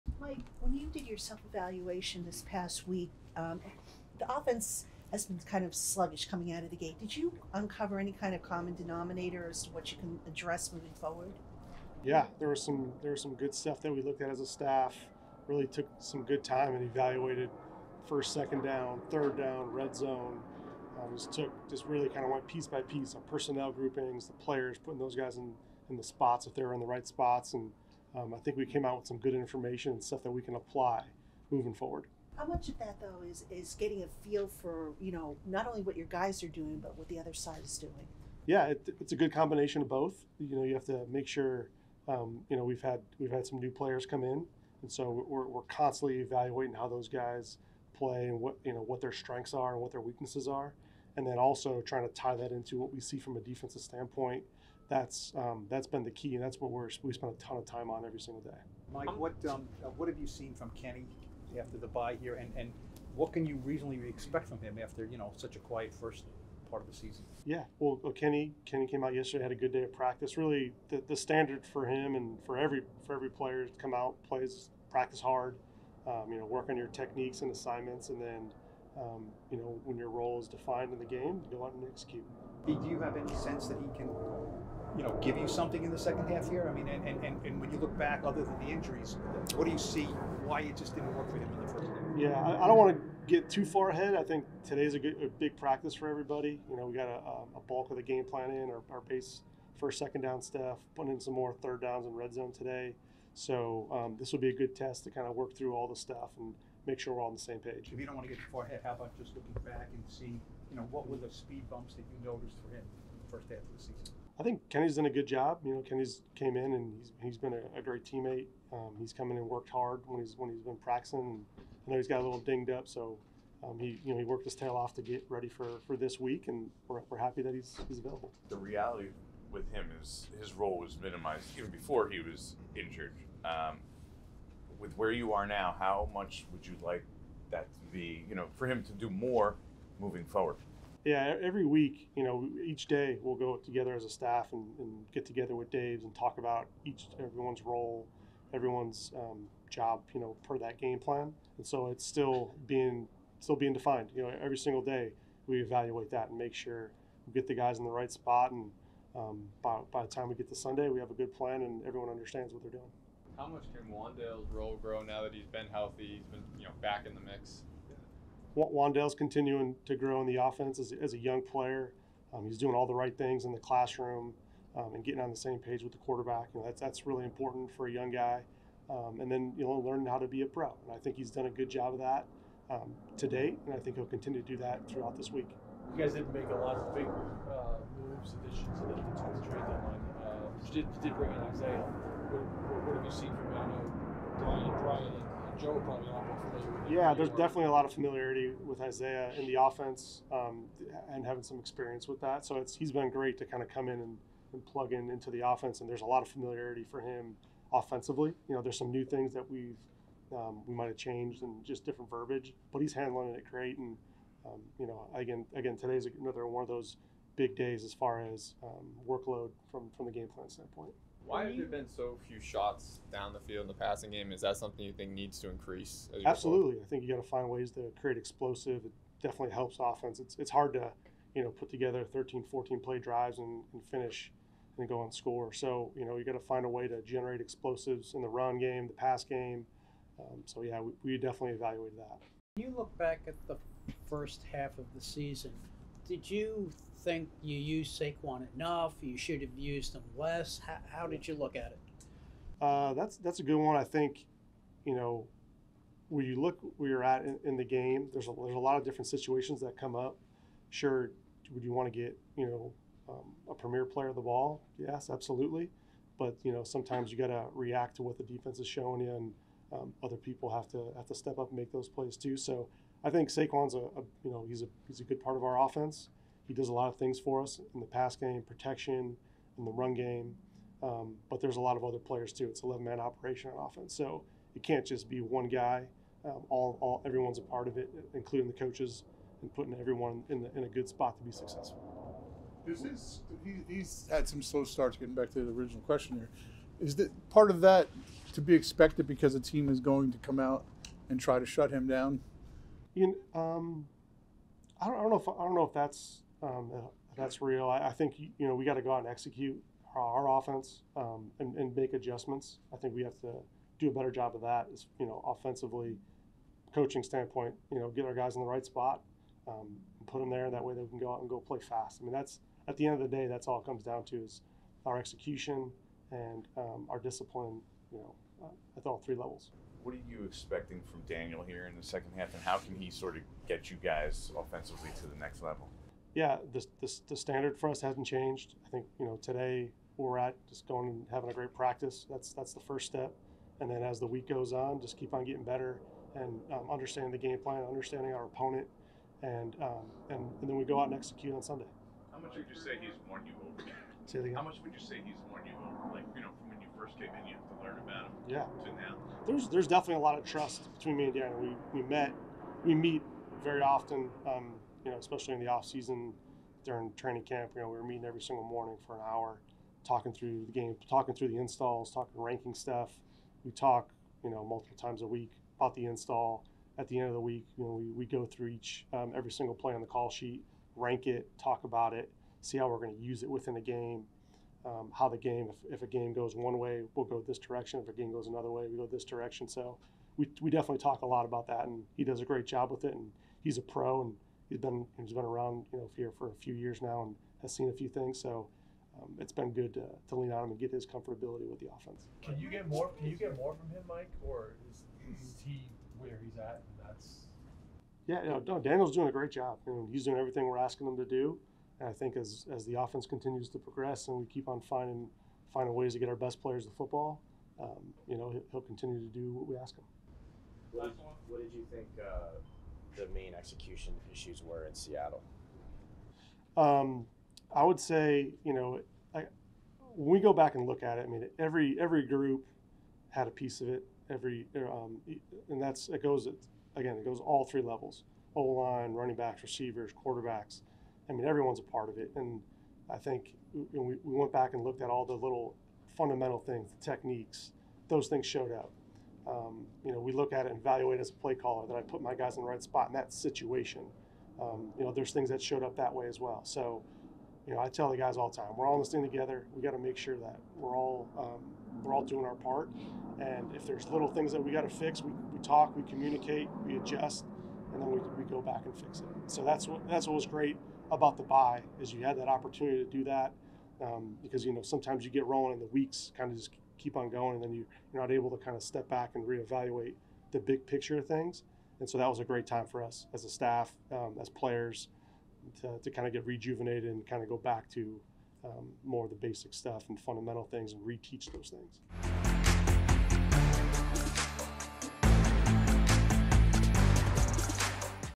Mike Kafka on Making Adjustments Following the Bye Week | New York Giants
Offensive Coordinator Mike Kafka speaks to the media before Wednesday's practice.